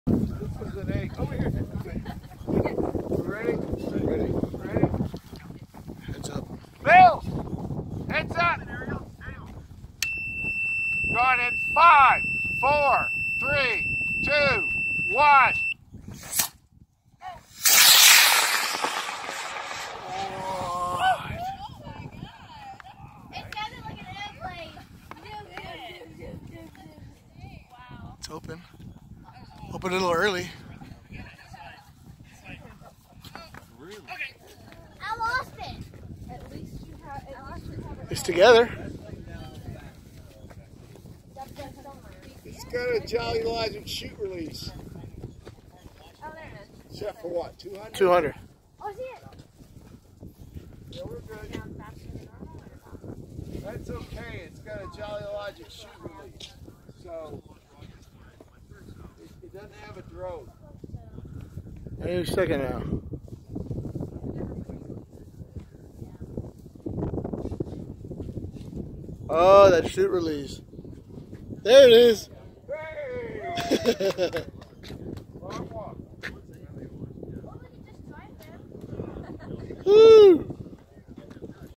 Hondo Launch - Black Brant Lvl 1 Cert H-100-10
Art Applewhite Hondo Launch. Black Brant level 1 certification launch on a H-100-10.  JLCR set to 200'